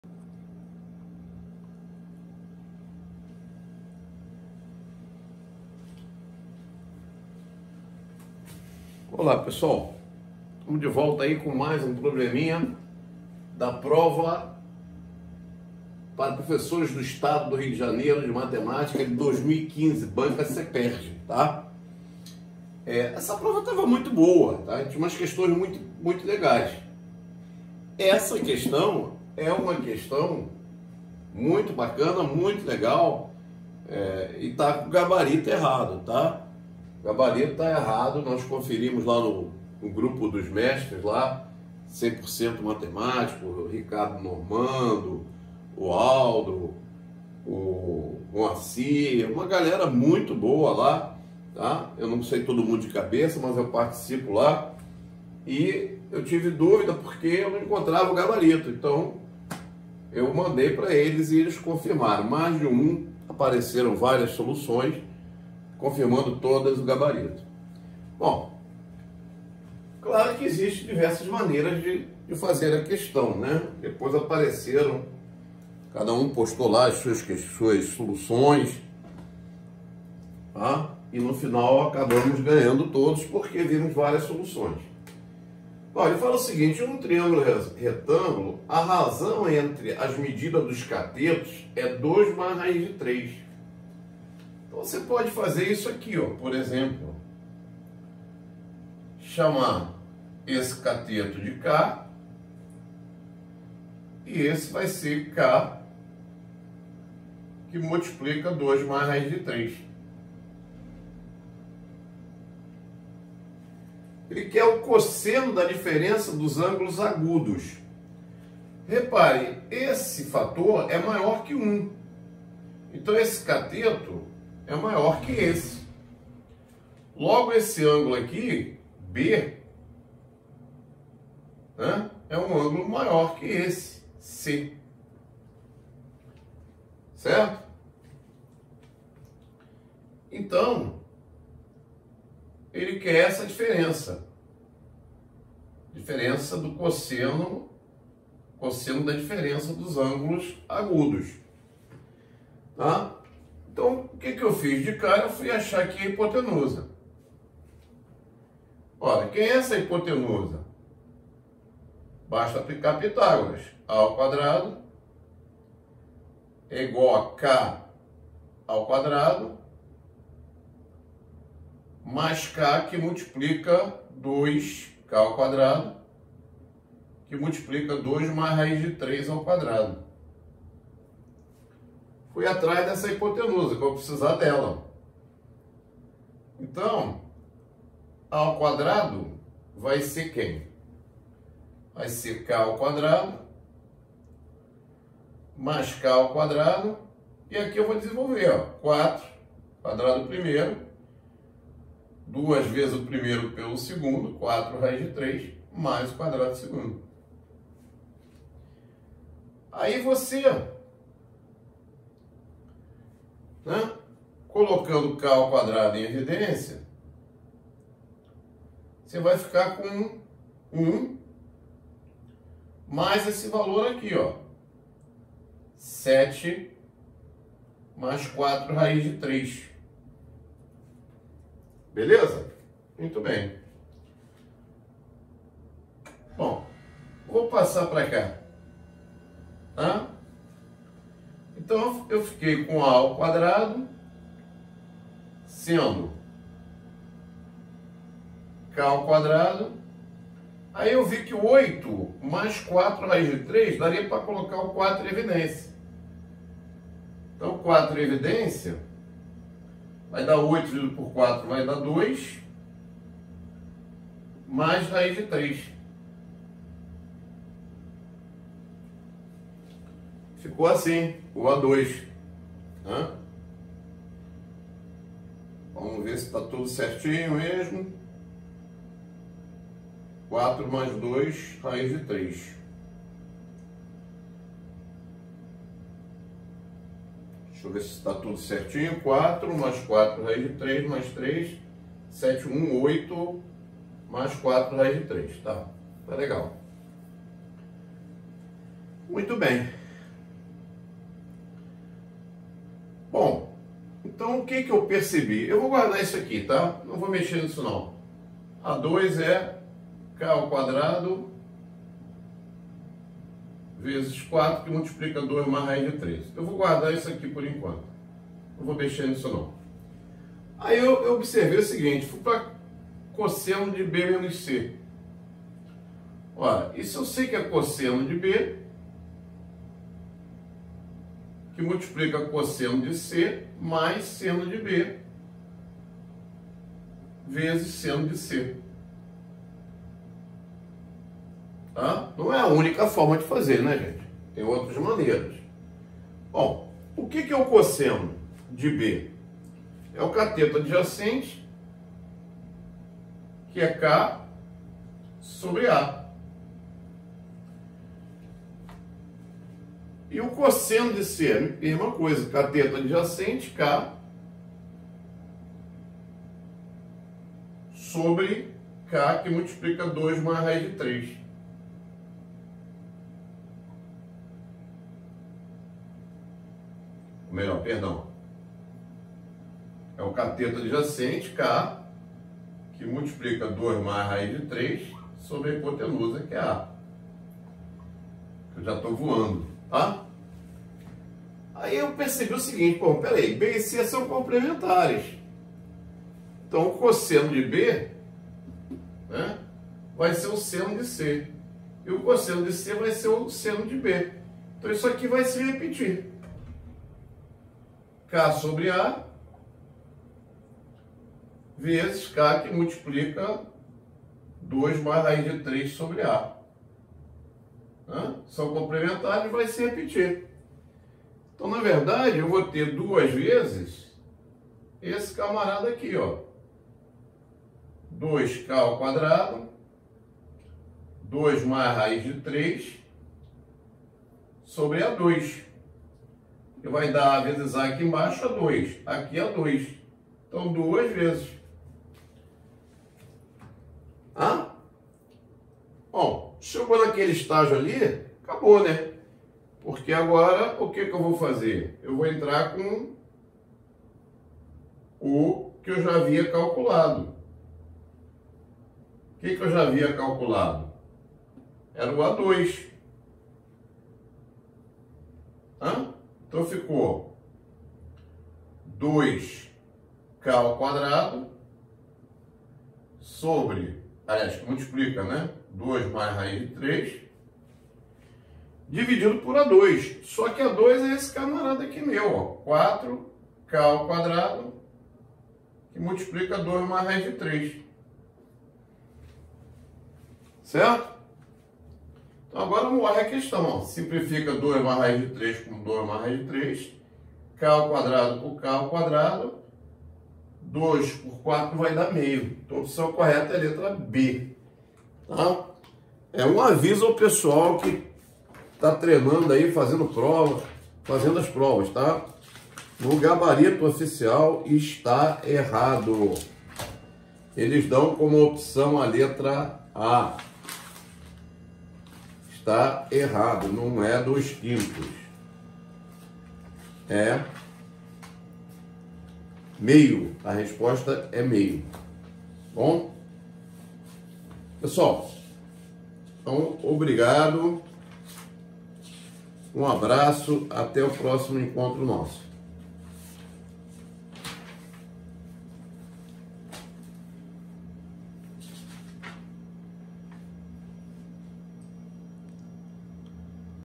Concurso Magistério -Estado RJ -Banca CEPERJ- Geometria Plana
Olá Pessoal

Trago mais uma questão para o concurso de professores da rede público. Trata-se de uma questão de Geometria Plana muito Legal. Espero que assistam e deixem seus comentários ao final .Obrigado